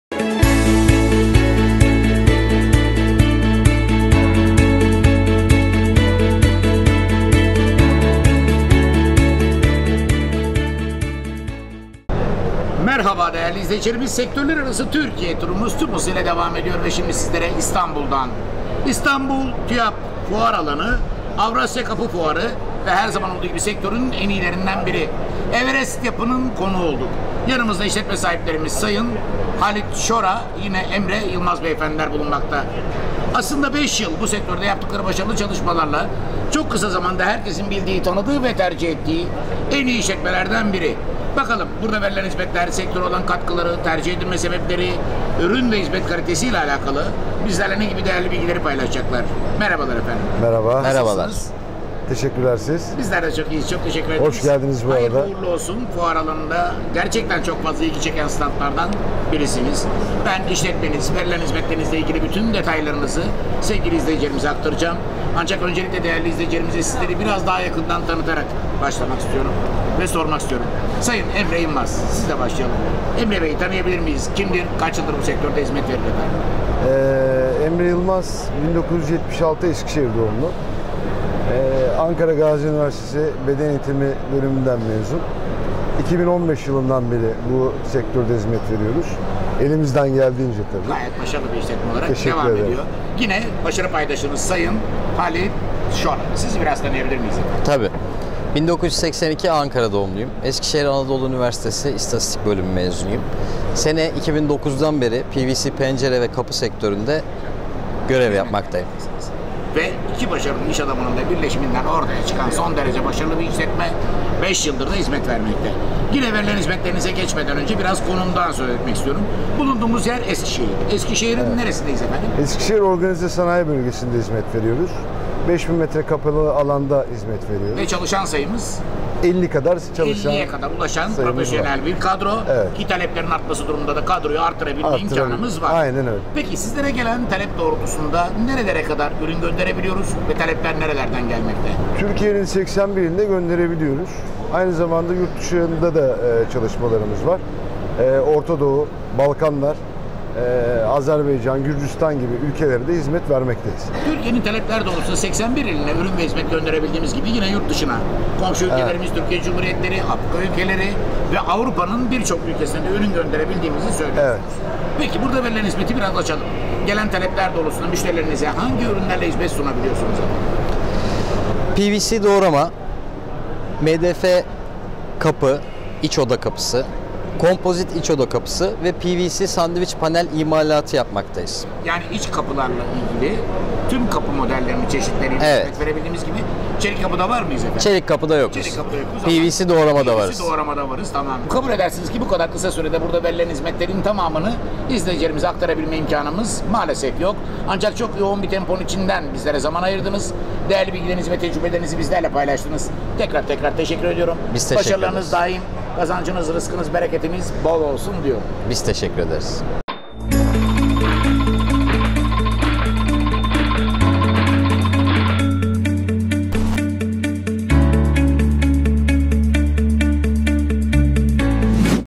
PORTETECH FUAR TV
Kobi O.S.B/Esekişehir'de faaliyetlerine devam eden sektörün önde gelen isimlerinden PORTETECH firmasının yetkilisi, firmanın standında muhabirimizin sorularını yanıtladı.